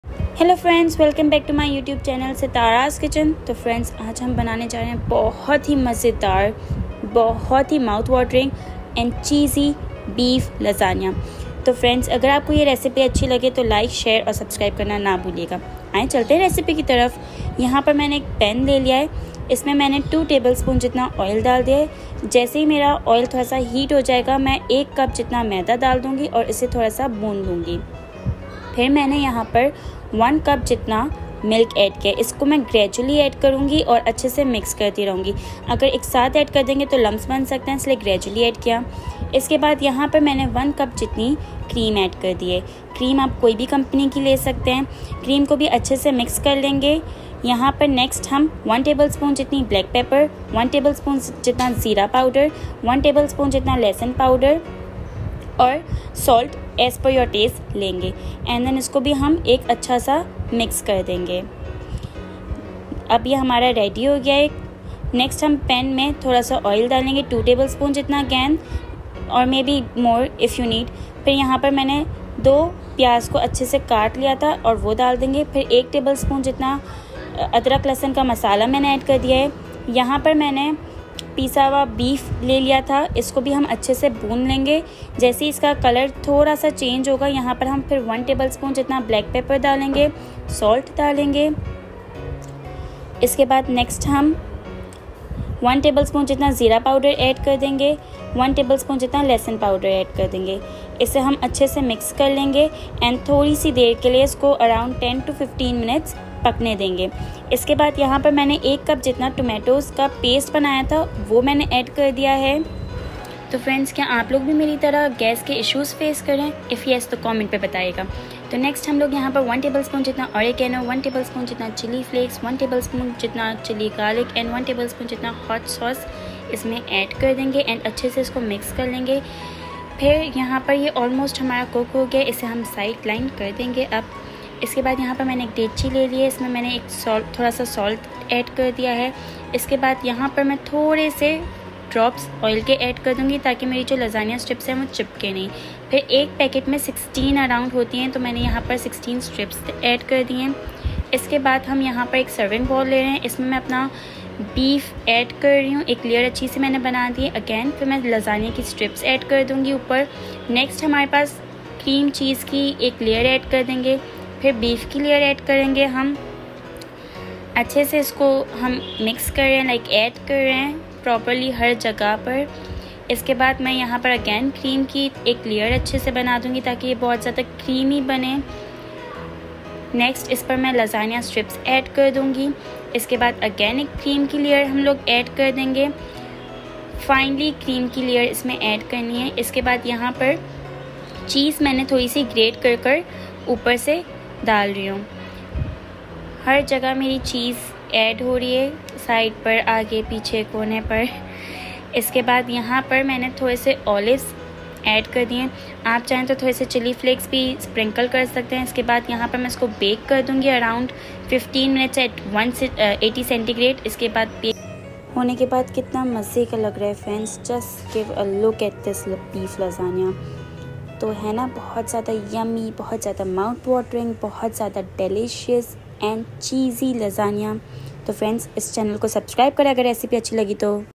Beef Lasagna | Delicious Cheesy Beef Lasagna Recipe By SitaraZ Kitchen
Ingredients to prepare cream:
- 2 tablespoons Oil
- 1 cup All-purpose Flour
- 1 cup Milk
- 1 cup Cream
- 1 tablespoon Salt
- 1 tablespoon Garlic Powder
- 1 tablespoon Black Pepper
- 1 tablespoon Oregano
- 1 tablespoon Cumin Powder

Ingredients to prepare beef:
- 1 cup chopped Onions
- Beef Mince (according to preference)
- 1 cup Tomato Paste
- 1 tablespoon Ginger Garlic Paste
- 1 tablespoon Cumin Powder
- 1 tablespoon Black Pepper
- 1 teaspoon Chili Flakes
- 1 tablespoon Garlic Powder

For Boiling Lasagna:
- Water (bring to boil)
- Salt
- Few drops of Oil
- Lasagna Sheets

Preparation:
1. Mix all the cream ingredients until a smooth consistency is achieved.
2. Mix all the beef ingredients and cook thoroughly.
3. In a baking dish, start layering with cooked beef.
4. Place lasagna sheets on top of the beef layer.
5. Add a layer of the prepared cream mixture.
6. Repeat layers of beef, lasagna sheets, and cream.
7. Sprinkle cheese between the layers as desired.
8. For the final layer, add lasagna sheets, remaining cream, cheese, chili flakes, and olives.
9. Bake in a preheated oven at 180°C for 15 minutes until the lasagna is cooked through.

Enjoy your delicious homemade beef lasagna!